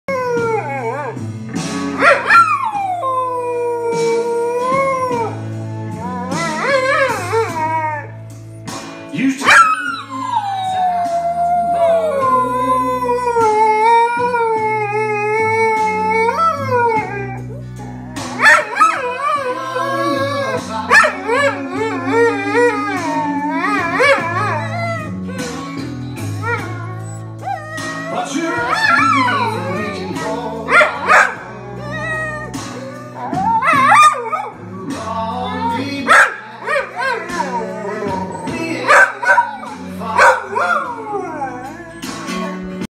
Tank singing with his sister Daphne!!
Daphne the bichon frise making her debut with her brother Tank the Bully!